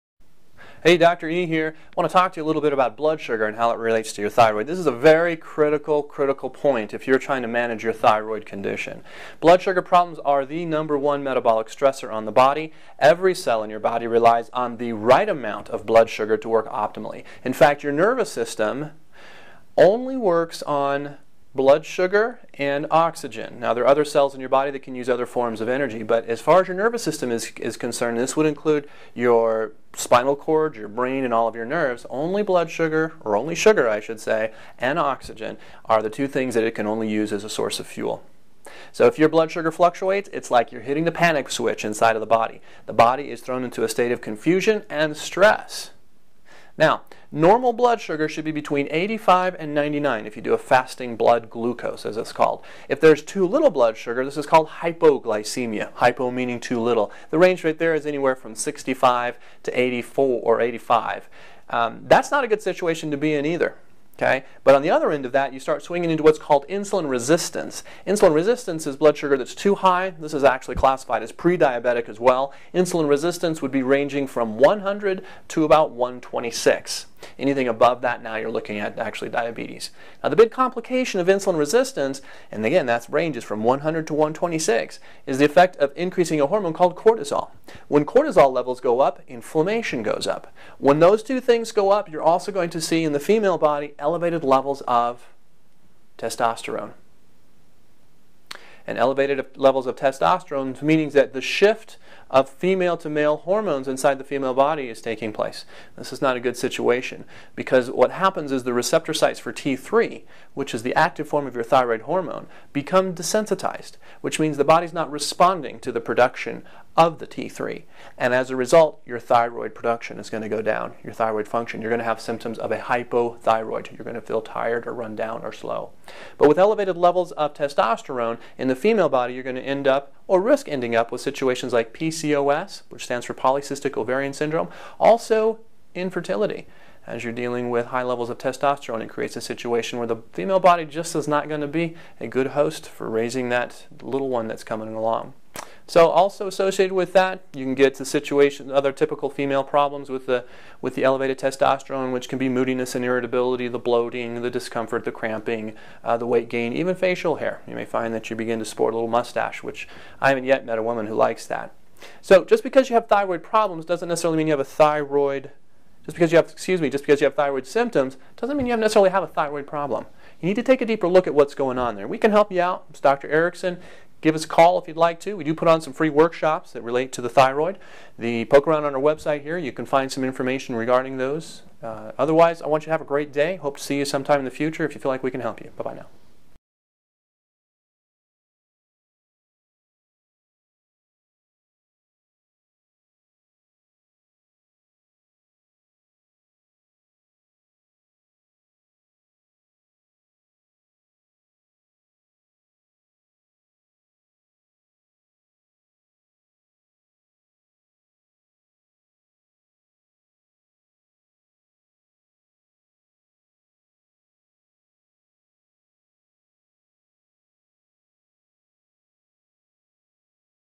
HAWC Ed: Your Blood Sugar Screws with your Thyroid
Dr. Erickson, DC discusses how even a little bit of a blood sugar problem will be a deal breaker in your efforts to fix your thyroid.